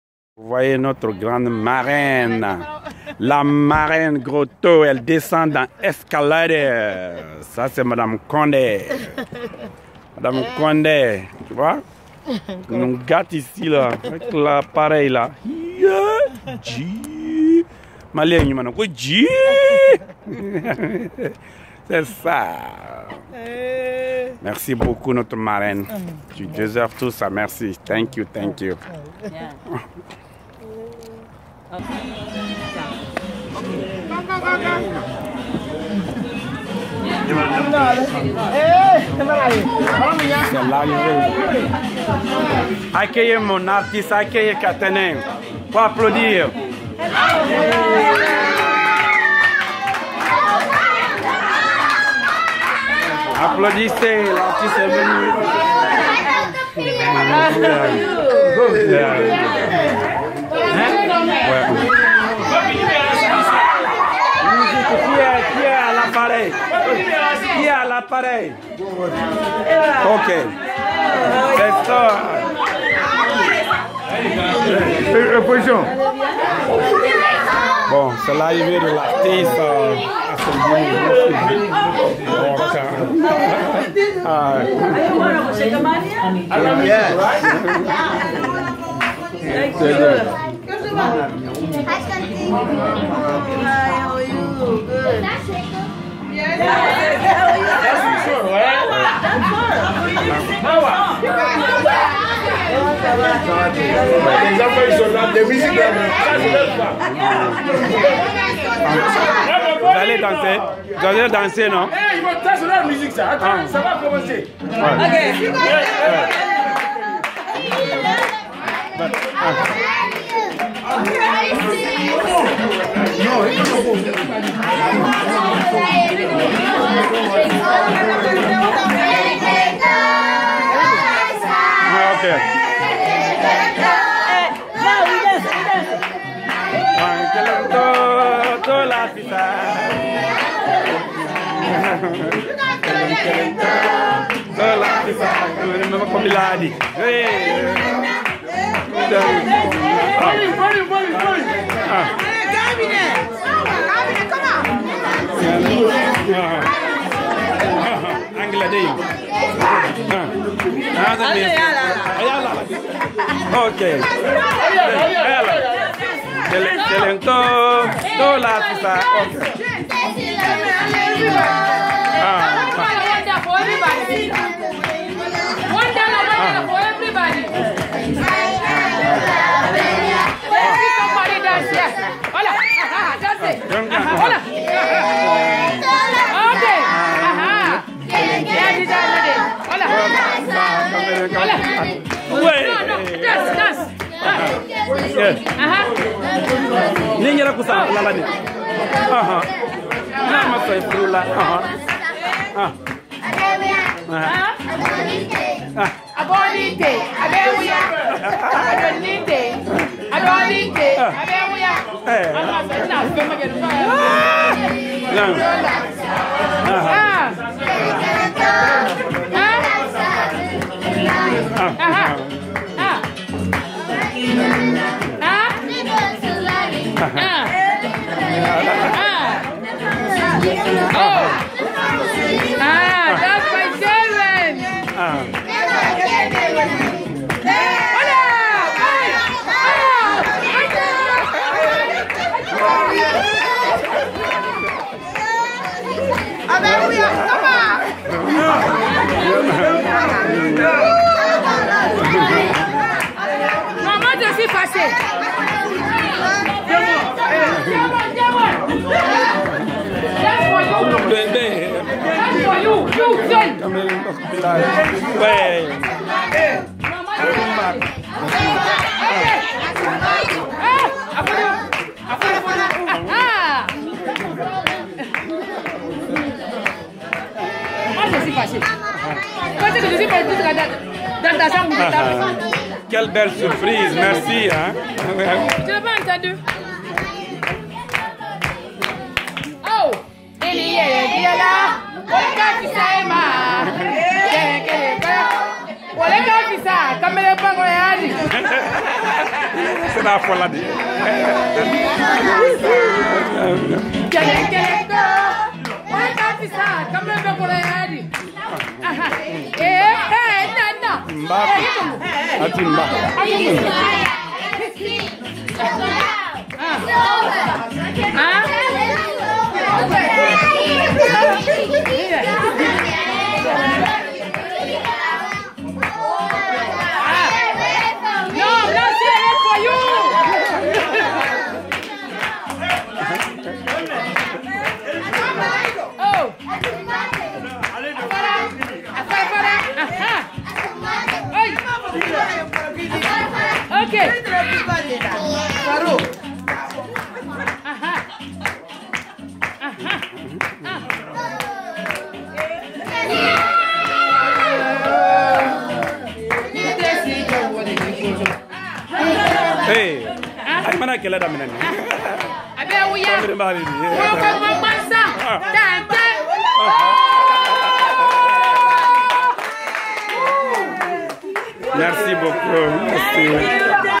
BEN SALIAN KOUYATE (DÎNER CHEZ PAPE FALL LE PARRAIN DE CHEKA)
le 11/27/2016 chez le parrain pape fall à columbus ohio.